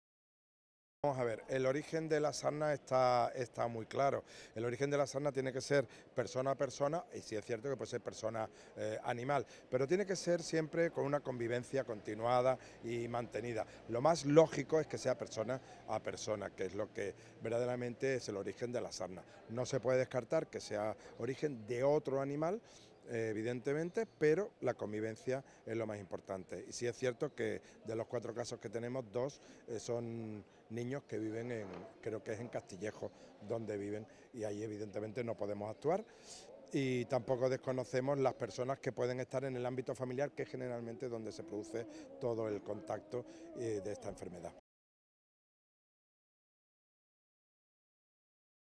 El consejero de Sanidad localiza el contagio de sarna a dos de los cuatro escolares en Castillejos
Situación controlada la de los cuatro casos de sarna detectados en dos colegios, manifiesta el Consejero de Sanidad, quien también advierte que, en dos de estos casos, el contagio se ha producido en la localoidad marroquí de Castillejos y se conoce cual ha sido el transmisor de la enfermedad: una gallina.